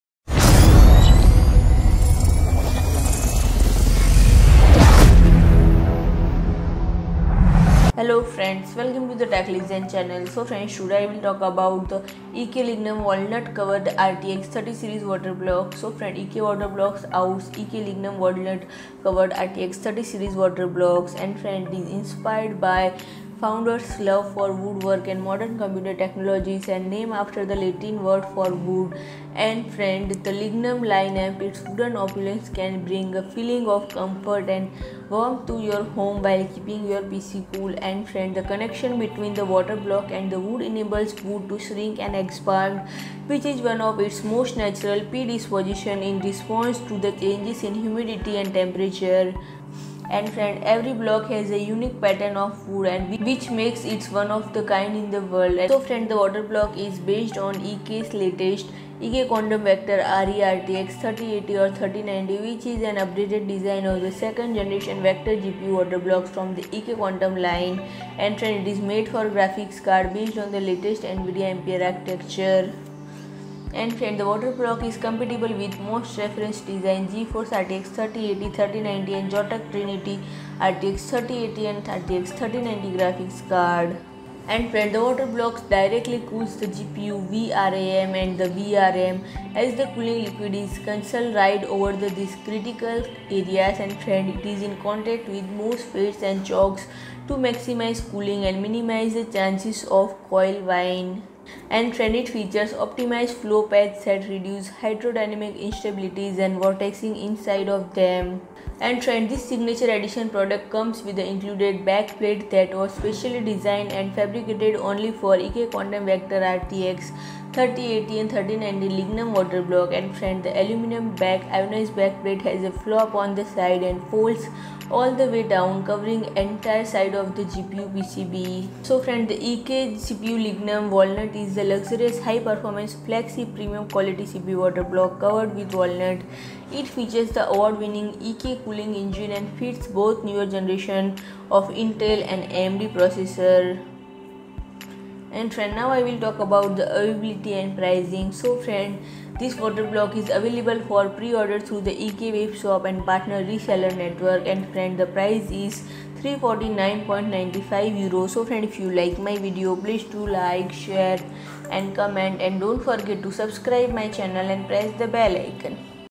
EK Lignum Walnut-covered RTX 30-series Water Blocks Covered By Walunt Launched | All Spec, Features
OFFICIAL WEBSITE TO ORDER :  COMING SOON

Similar Products :-
EK, the leading premium liquid cooling gear manufacturer, announces the launch of a new addition to the Signature Edition product portfolio - the EK Lignum. It is inspired by our founder's love for woodwork and modern computer technologies and named after the Latin word for wood. A perfect symbiosis of the oldest natural building material and contemporary high-end cooling solutions. An unparalleled example of how your PC can reflect your lifestyle. The Lignum line and its wooden opulence can bring a feeling of comfort and warmth to your home while keeping your PC cool and silent for its maximum possible performance. The choice of wood is walnut (Juglans Nigra) because it never fails to surprise with its colors and texture. The connection between the water block and the wood enables wood to shrink and expand, which is one of its most natural predispositions in response to the changes in humidity and temperature. Lignum products were engineered in a way so that the wood does not come into direct contact with water. Every block has a unique pattern of wood, which makes it one of a kind in the world. The Lignum GPU water block at hand is based on EK's latest EK-Quantum Vector RE RTX 3080/3090, which is an updated design of the 2nd-generation Vector GPU water blocks from the EK-Quantum Line. It is made for graphics cards based on the latest NVIDIA Ampere architecture. The EK-Quantum Vector RTX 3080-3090 Lignum water block is compatible with most reference design (not Founders Edition) GeForce RTX 3080, 3090, and Zotac Trinity RTX 3080 and 3090 graphics cards. It is purpose-designed with a much thicker copper base to clear the tall capacitors on most reference design graphics cards. This gives the water block the advantage of having a wider compatibility list without sacrificing any coolant flow. This extended-compatibility water block fits even more reference PCB designs, but for a precise compatibility match of this water block, we recommend you refer to the EK Cooling Configurator. The water block directly cools the GPU, VRAM, and the VRM (voltage regulation module) as the cooling liquid is channeled right over these critical areas. It is in contact with MOSFETs and chokes to maximize cooling and minimize the chances of coil whine. These newly developed water blocks feature optimized flow paths that reduce hydrodynamic instabilities and vortexing (dead spots) inside of them. Additionally, this new design allows the terminal to be screwed directly onto the copper cold plate of the GPU block, making it more rigid and reducing the chances of damaging the water block. This Signature Edition product comes with an included backplate that was specially designed and fabricated only for the EK-Quantum Vector RTX 3080/3090 Lignum water block. The aluminium black anodized backplate has a flap on the side that folds all the way down, covering the entire side of the GPU PCB. You can also explore other Lignum line products to build a PC with a perfect blend of nature and technology. The EK-CPU Lignum - Walnut is the luxurious high-performance flagship premium quality CPU water block covered with walnut. It features the award-winning EK cooling engine and fits both newer generations of Intel and AMD processors. EK-HDC Lignum 12 mm - Walnut is the hard tube fitting made to blend perfectly with EK-Lignum water blocks. It is made for 12 mm (Outer Diameter) hard tubing. Every single piece of wood is carefully chosen, handcrafted, and varnished with multiple layers. The open-pore matte varnish gives the wood its natural silky touch. It is attached to the acetal tops, which seal the block and act as functional tops, while wood acts as an aesthetical one, meaning there is no contact of wood with liquid in the custom loop. The EK Lignum product line is now available worldwide through a network of partners and resellers, as well as directly through the EK Online Store.